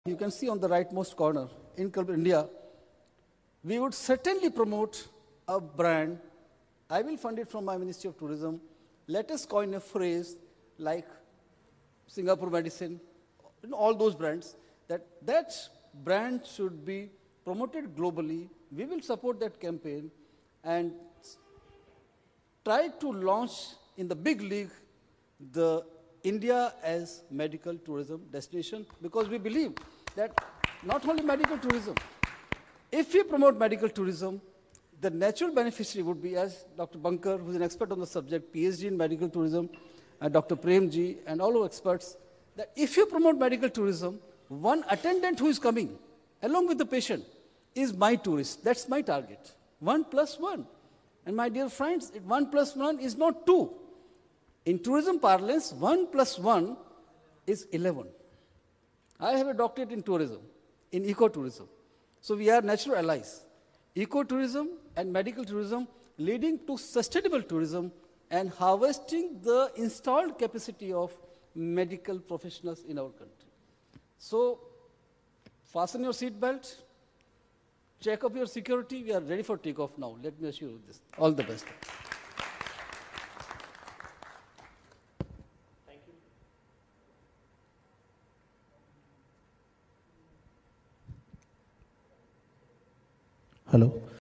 Dr Lalit K Panwar Optimistic About Medical Tourism in India
International India Medical Tourism Congress (IIMTC) – ‘A journey for Health’, recently held in Delhi from December 13 to 14, 2014,

Addressing the delegates, Dr Lalit K Panwar, secretary, ministry of tourism (MoT), Government of India, talked about“We should look into creating a global brand like ‘Incredible India’ for medical tourism also and launch an international media campaign to promote India as an affordable but world class medical center and facilitator.”